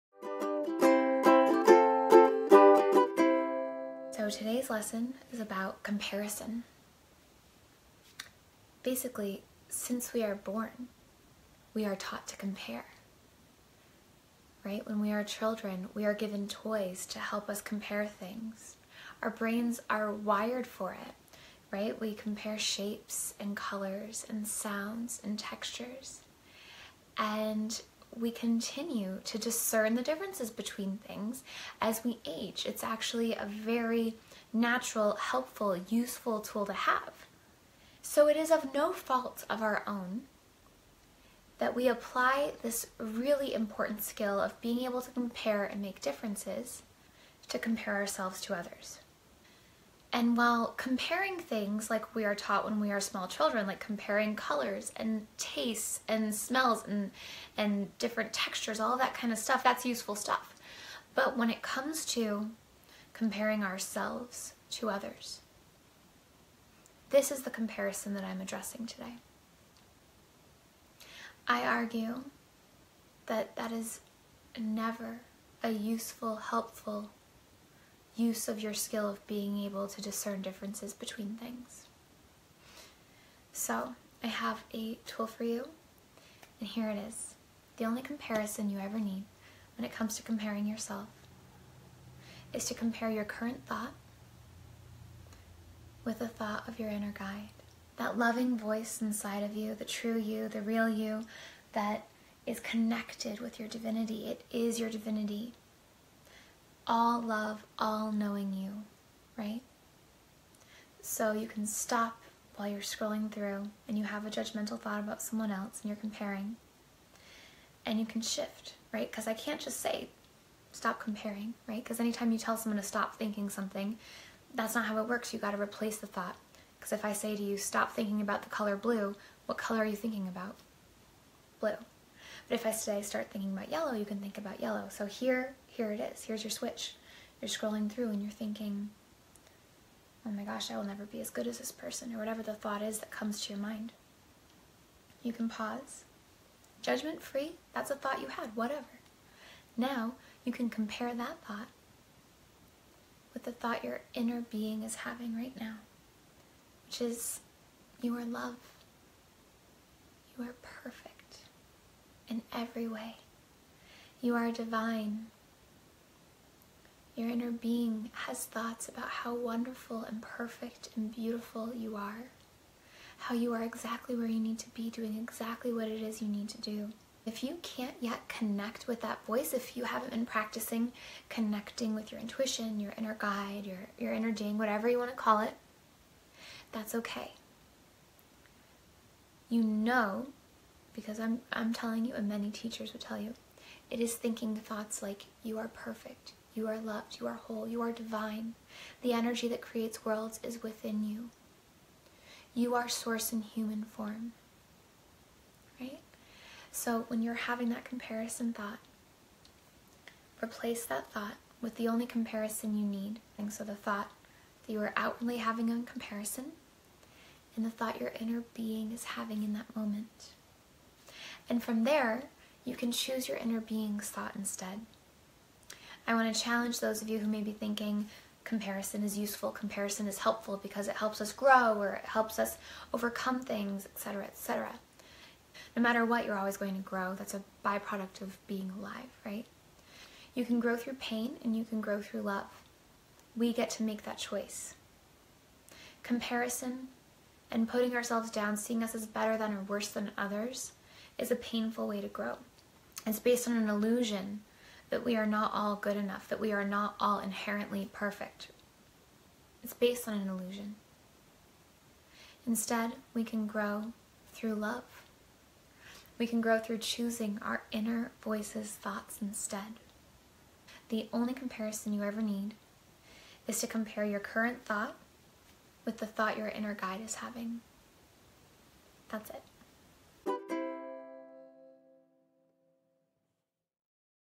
The Only Comparison You Need
Mondays with Marisa, Episode 1: The Only Comparison You Need

In this episode, Marisa shares how detrimental comparing ourselves to others can be and instead offers a simple switch to break the habit.